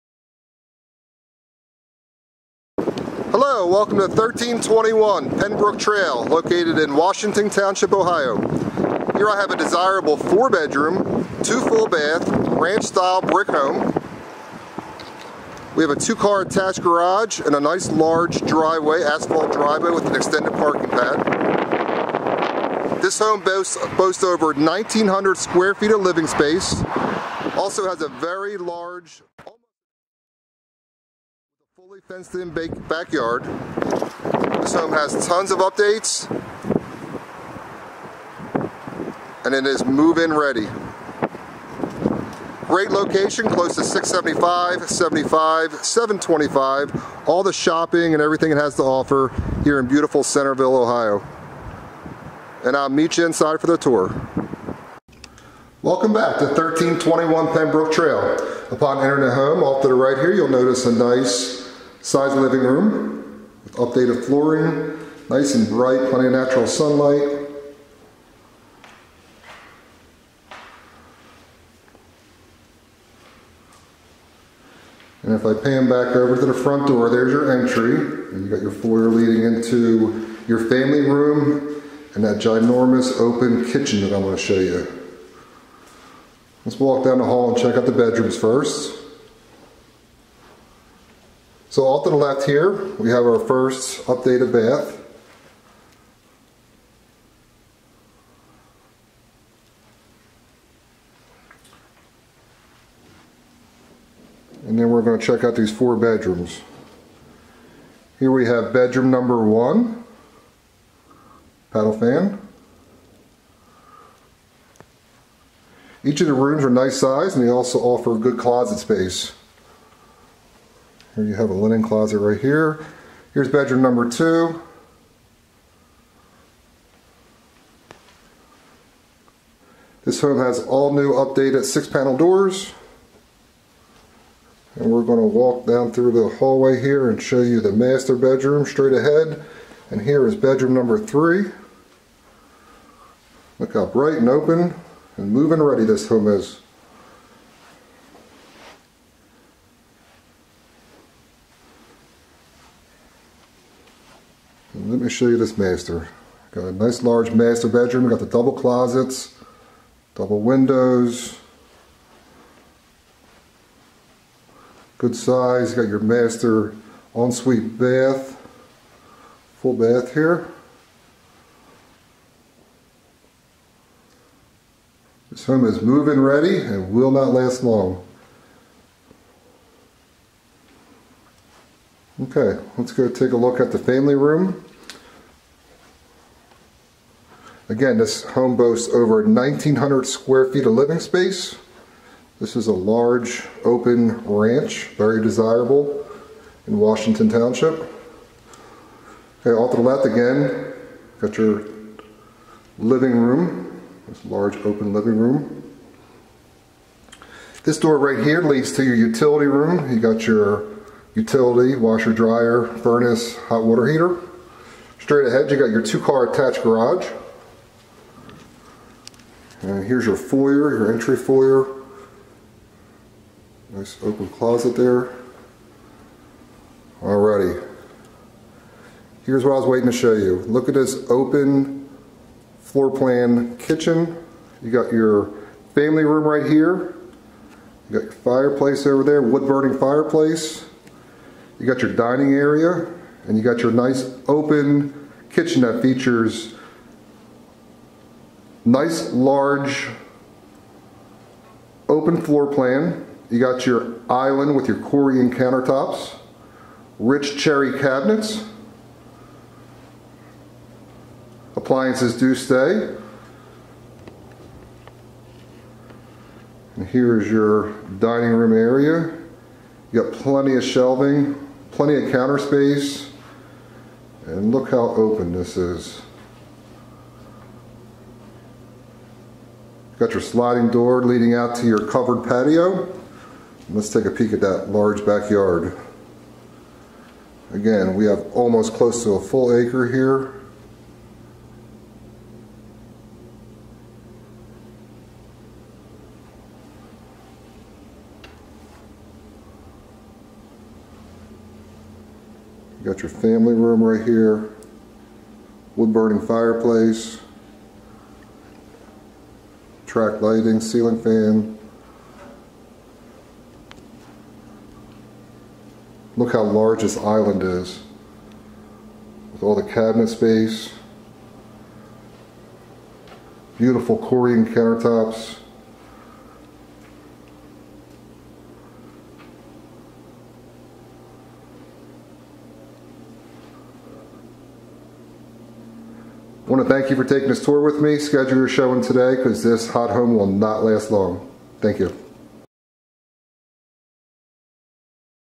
SOLD! 1321 Penbrooke Trail Washington Township, OH 45459 | For Sale | 4 bedrooms, 2 Baths
4 Bedrooms | 2 Baths | 2 Car Attached Garage | 0.51 Acres - $249,900

Your search is over with this beautiful move in ready 4-bedroom brick ranch set on a large lot. This home features an open kitchen with rich cherry cabinetry, Corian countertops, and an oversized island. The adjacent dining area and family room with corner wood burning fireplace offers a great place to entertain guests. A large master suite features 2 closets and updated bath. 2 car attached garage and shed offers plenty of space for storage. A large extended pad driveway offers ample parking for guests. Fenced in backyard with mature trees and covered patio. Enjoy outdoor activities at nearby Yankee Park. Great location with quick access to 75, 675, 725 and shopping. This is a great family home at a great price. Schedule your showing today before it’s too late.

Welcome to Washington Township, Ohio
Welcome to Centerville Schools
Welcome to Centerville Elementary Schools
Welcome to Centerville Middle Schools
Welcome to Centerville High School